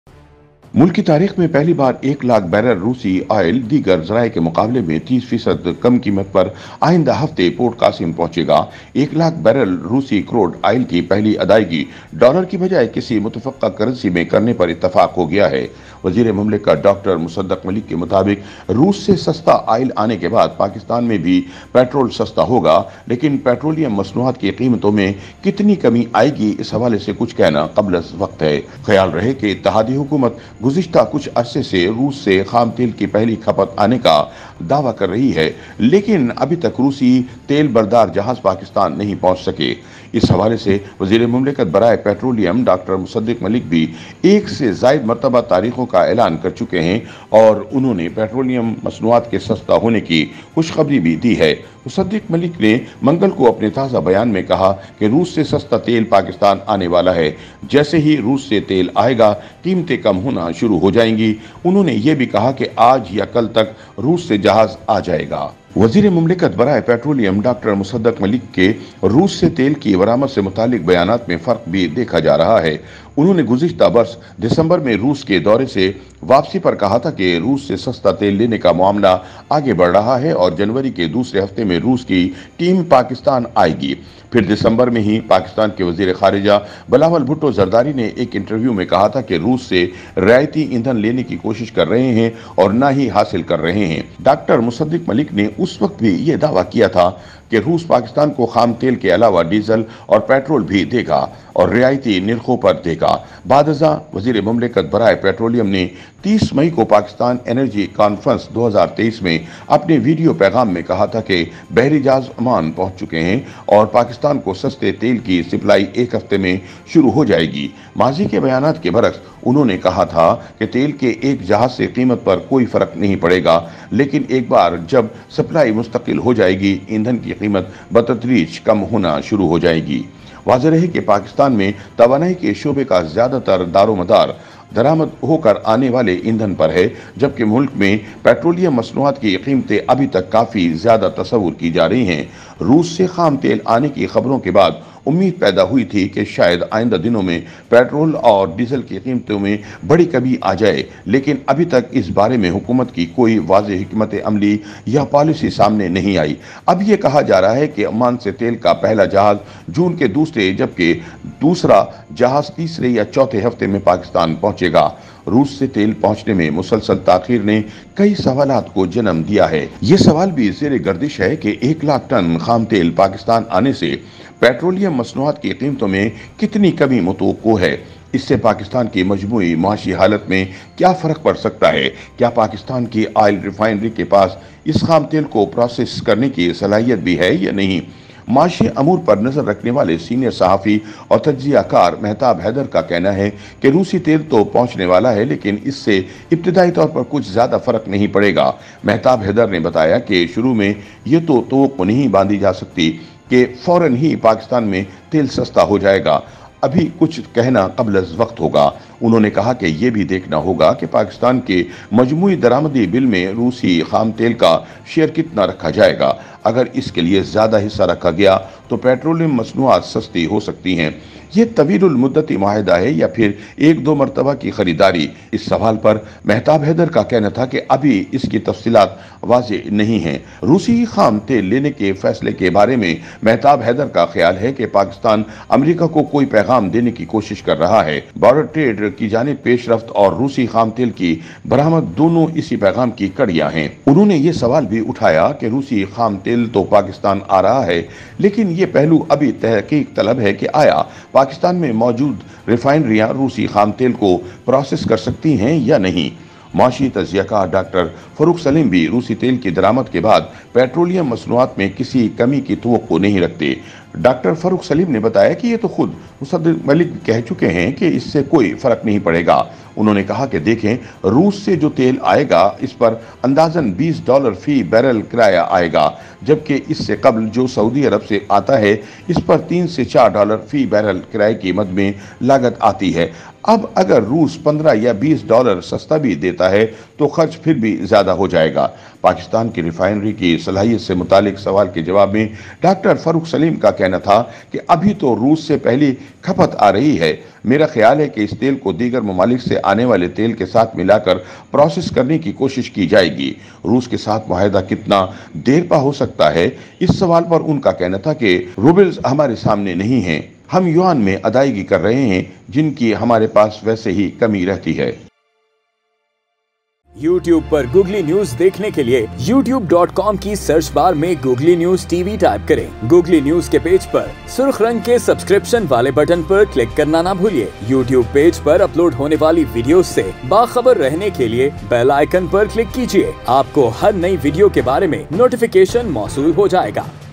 Russian Oil Kei Import Sai Pakistani Petrol Kitna Sasta Ho Ga? | Googly News TV
Russian Oil Kei Import Sai Pakistani Petrol Kitna Sasta Ho Ga?